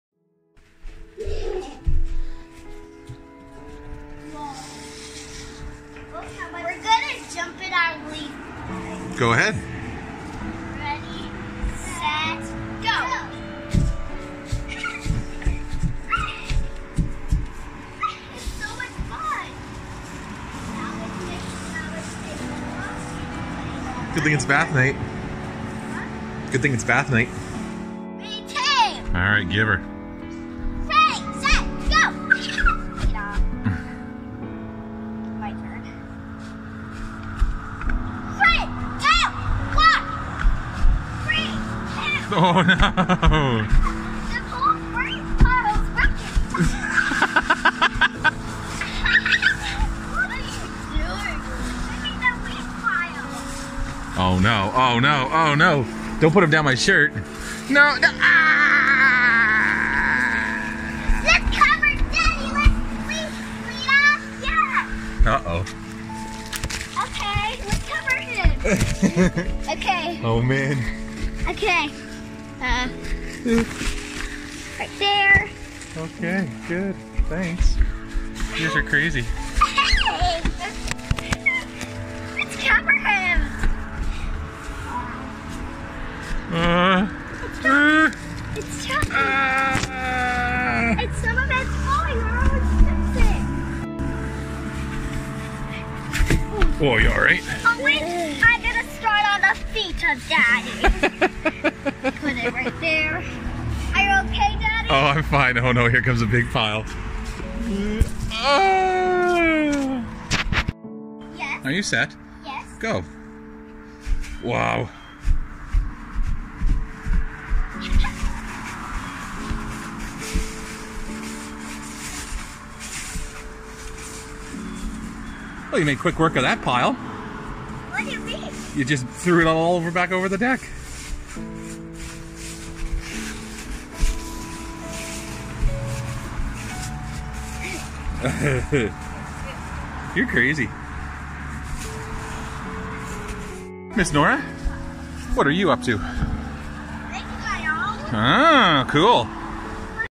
The Leaves be Fallin!
nothing beats some play time in a fresh leaf pile!